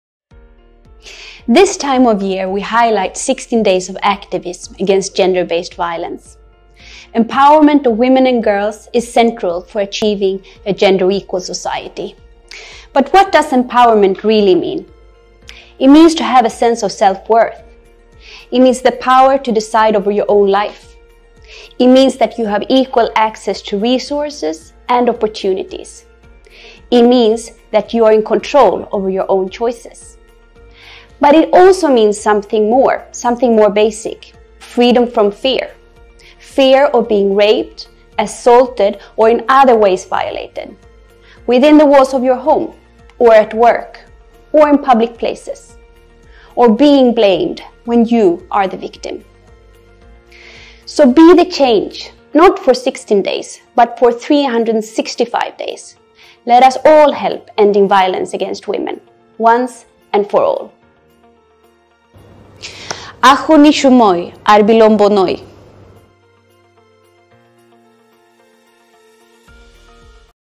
Alexandra Berg von Linde | Ambassador of Sweden to Bangladesh
🚺 Gender-based violence is a barrier to women's full integration into the economy and limiting economic prosperity. Be the CHANGE. Not for 16 days – but for 365 days. 🧡🎗️🇺🇸🇸🇪🇧🇩

Thanks to Ambassador Alexandra Berg von Linde, Embassy of Sweden in Dhaka for supporting our joint call to action. 𝐍𝐨 𝐓𝐢𝐦𝐞 𝐭𝐨 𝐖𝐚𝐬𝐭𝐞, 𝐈𝐭’𝐬 𝐓𝐢𝐦𝐞 𝐭𝐨 𝐀𝐜𝐭